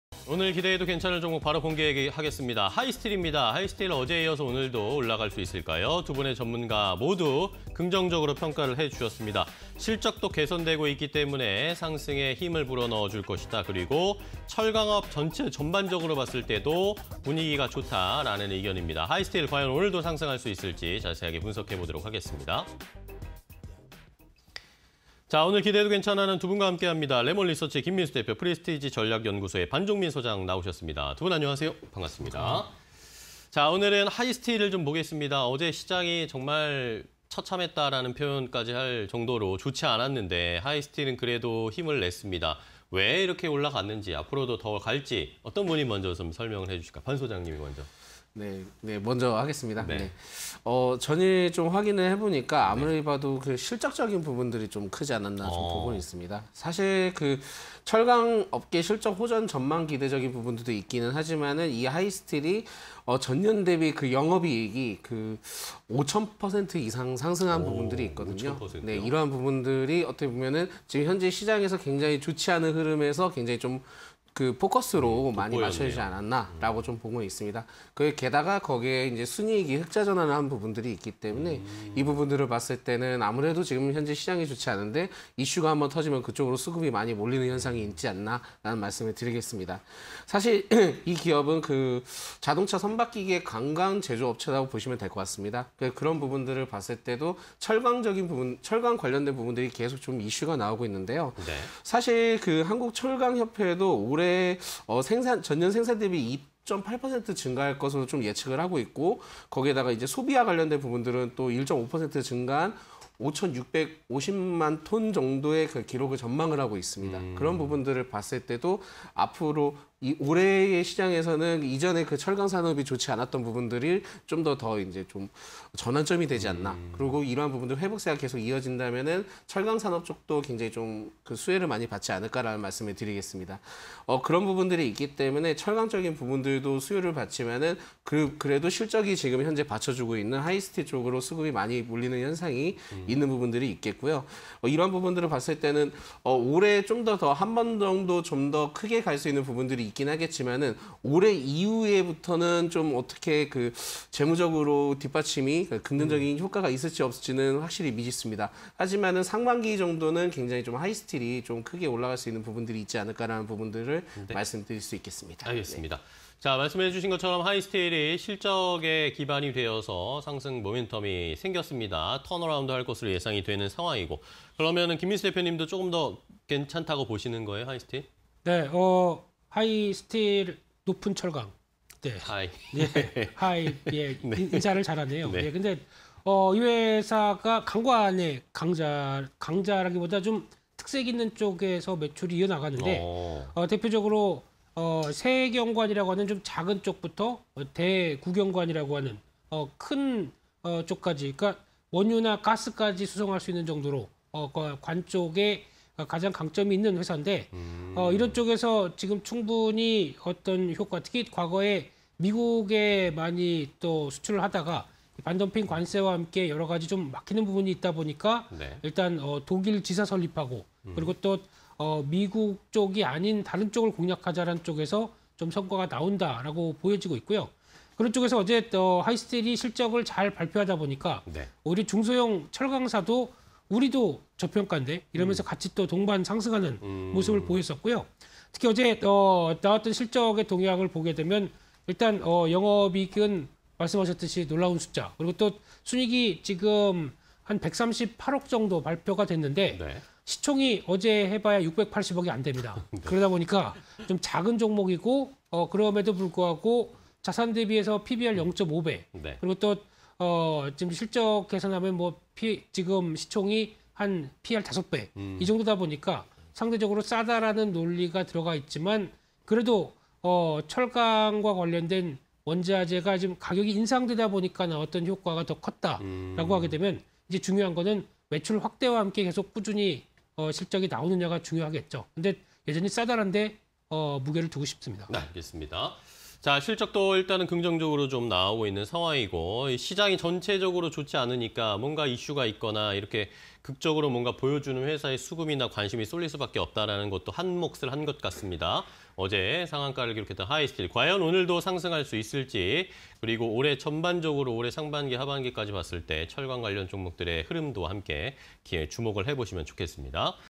하이스틸... 실적 개선, 철강업 실적 호전 기대_기대해도 괜찮아 (20220126)
하이스틸 더 간다
- 금일 추가 상승 가능성有
- 강관 전문 제조
- 철강업 실적 호전 전망 기대
- 국내 철강 수요 증가 및 공급 안정화
- 작년 영업익 164억… 전년비 5,561%

# 하이스틸 더 간다
- 실적 개선
- 한일철강 인적분할… 강관 주력
- 美 반덤핑 관세 등 보호 무역 강화
- 독일 지사 설립 및 비미주 시장 공략
- 지난해 호실적… 흑자전환